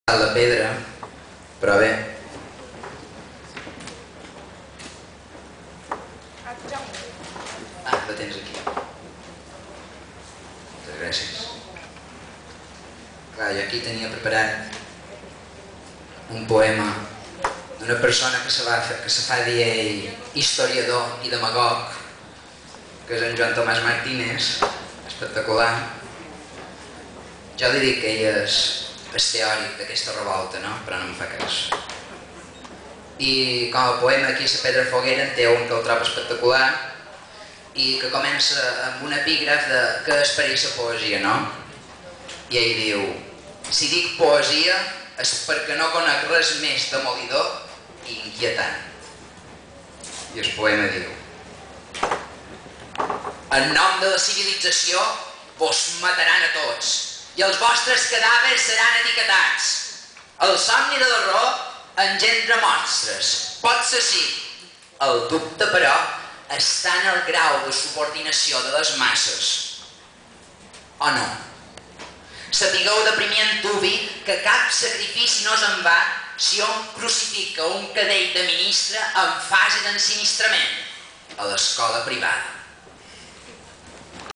Jaume C. Pons Alorda i Blanca Llum Vidal a la Setmana del Llibre en Català (7/15)
Recital en motiu de la presentació de La cabra que hi havia de Blanca Llum Vidal i Els estris de la llum de Jaume C, Pons Alorda, a la 20a Setmana del Llibre en Català de Palma, divendres dia 13 de març de 2009, al Pati de la Misericòrdia.